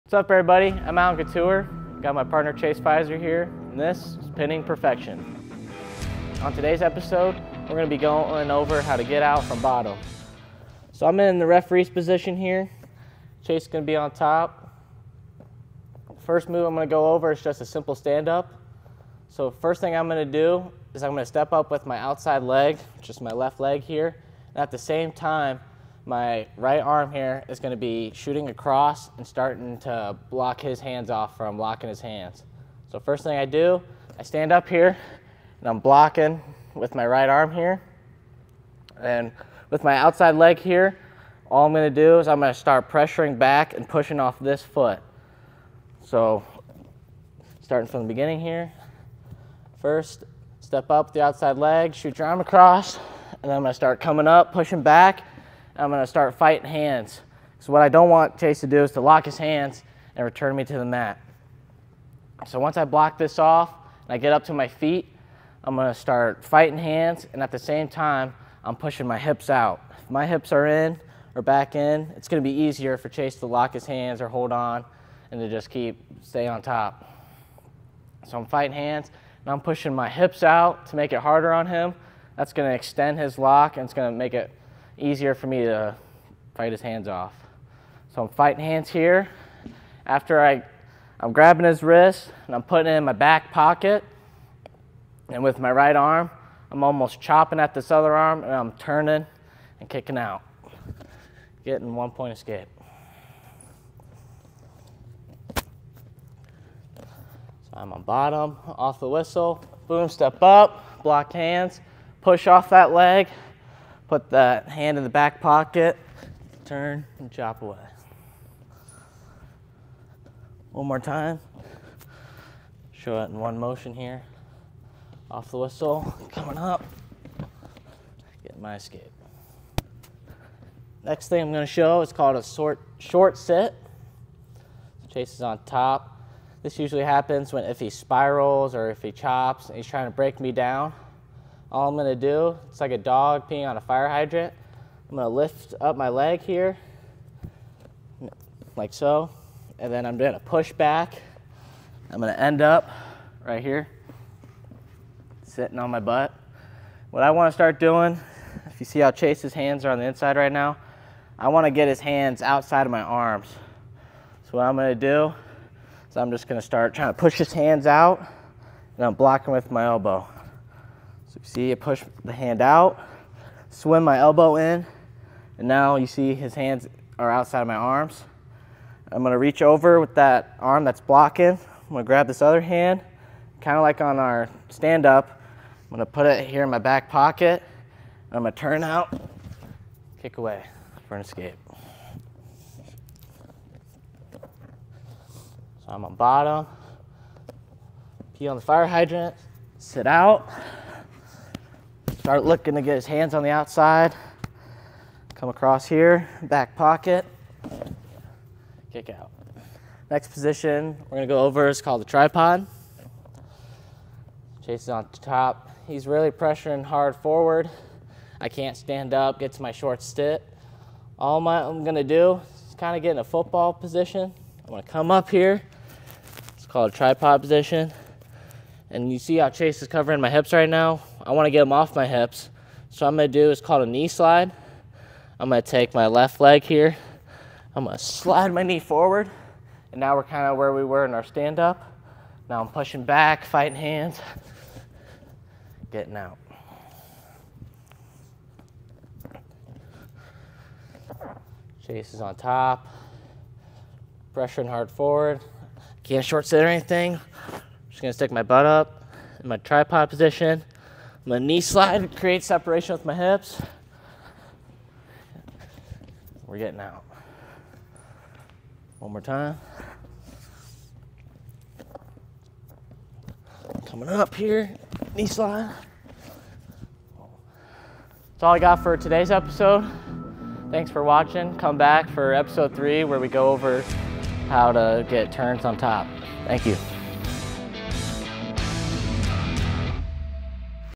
Pinning Perfection- Episode 2 (Bottom Escapes)
Throughout this wrestling technique series, Duhawk wrestler Allen Catour takes you through the three main areas of wrestling: neutral, bottom, and top. In this episode, Allen goes through scoring escapes from the bottom position.